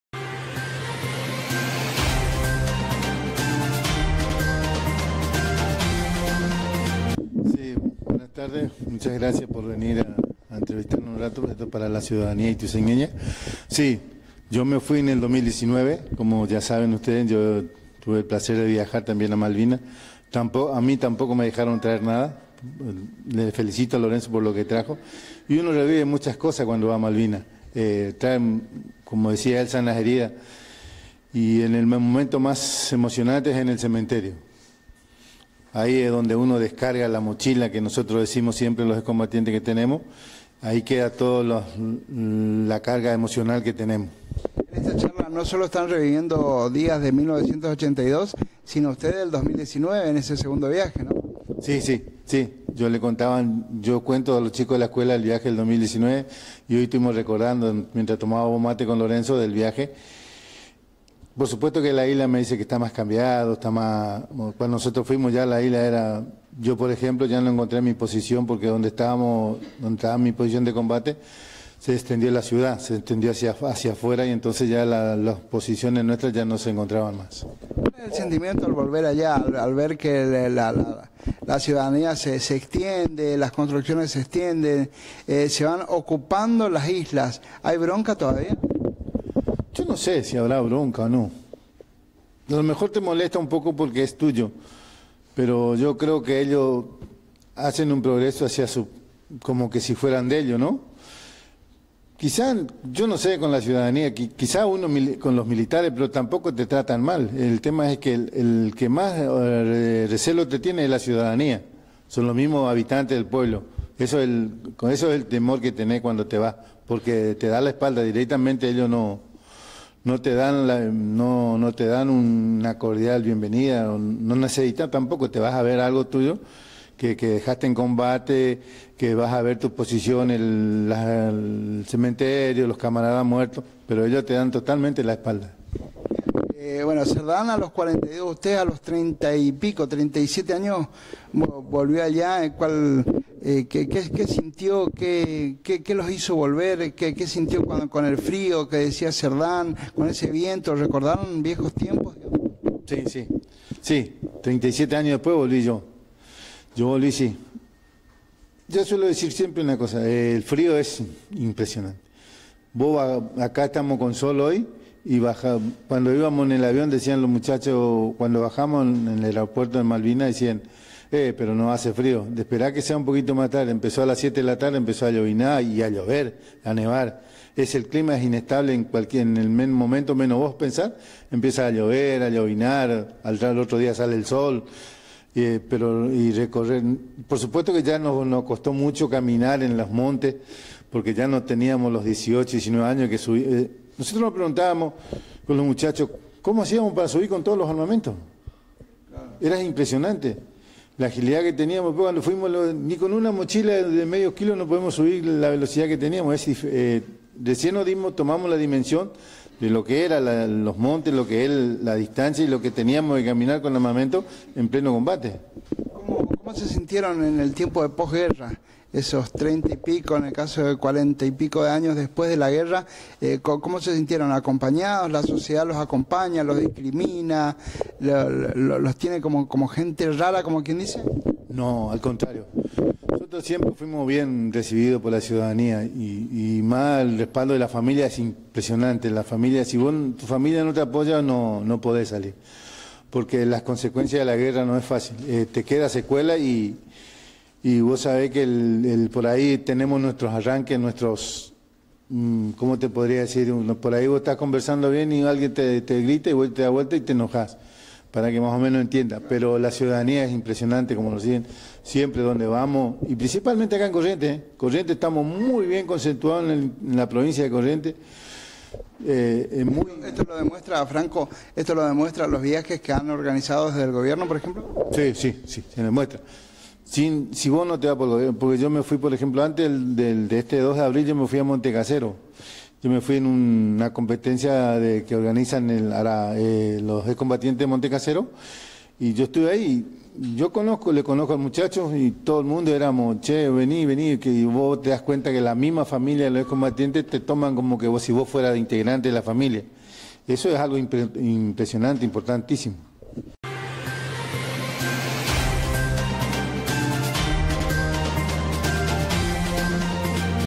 RAMON FRANCO EX COMBATIENTE E MALVINAS
Franco tambien volvió a las Islas hace algunos años e intercambió Charla con Lorenzo Cerdán